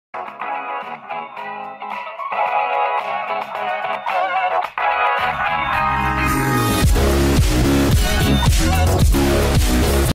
My intro
Today I'm uploading my intro for my channel. I'm also looking for somebody to make me one if possible thank you guys!